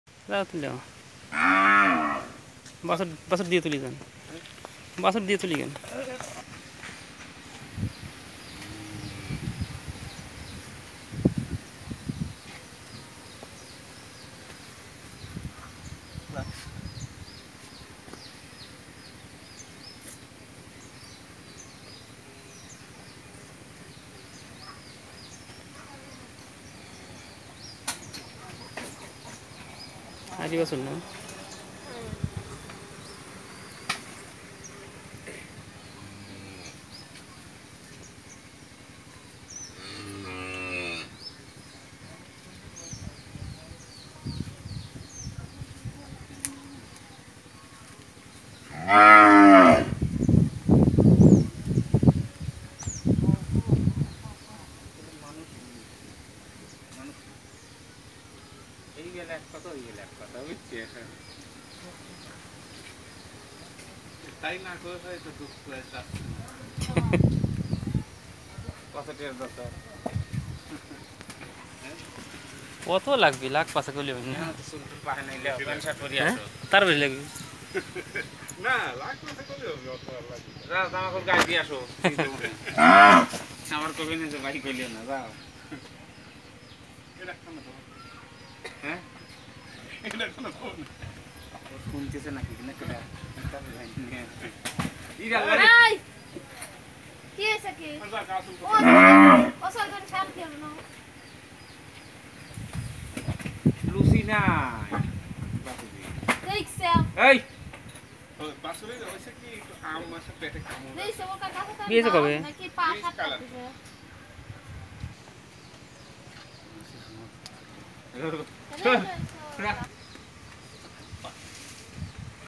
১৬৬২ নং// ১ম বিয়ানের ফ্রিজিয়ান জাতের গাভী বিক্রয় করা হইবে। 01710135019// 01884383400
এটি একটি ফ্রিজিয়ান জতের গাভী। গাভীটি এবার ১ম বার বাচ্চা প্রসব করেছে। গাভীটি একটি ষাঁড় বাচ্চা প্রসব করেছে। গাভীটির সকাল বিকাল দুধের পরিমান ১৫-১৬ লিটার।

      গাভীটি পারিবারিক ভাবে লালন পালন করা। গাভীটি একদম রোগ বালাই মুক্ত। জাত ও মানের দিক পর্যালোচনায় এই গাভীটি বাংলাদেশ ডেইরী শিল্প উন্নয়নের উচ্চ ক্ষমতা সম্পন্ন।
দামঃ ১,৫৫,০০০/= টাকা।

যোগাযোগের ঠিকানাঃ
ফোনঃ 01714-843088
পাবনা, বাংলাদেশ ।